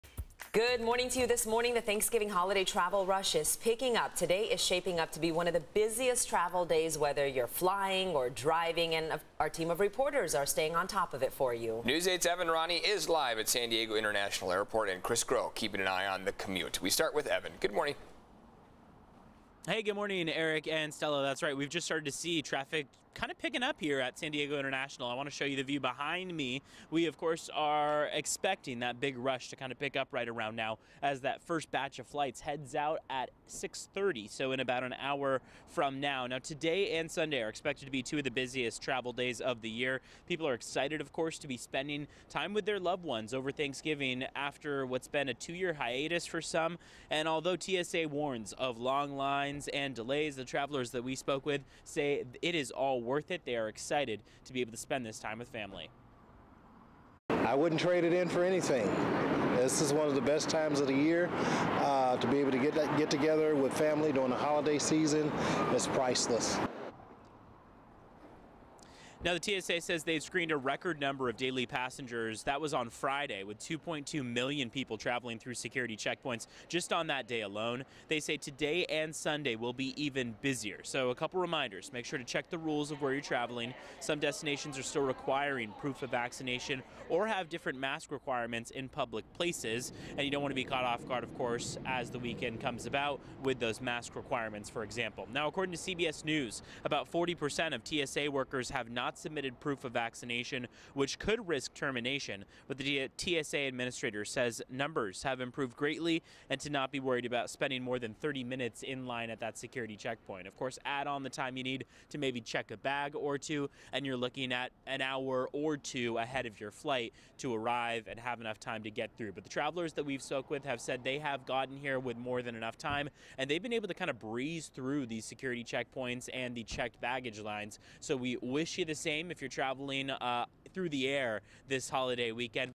Holiday travel at the airport picking up as we get closer to Thanksgiving
Airports across the country bracing for about 2 million passengers a day between Wednesday and Sunday, the most since the pandemic started.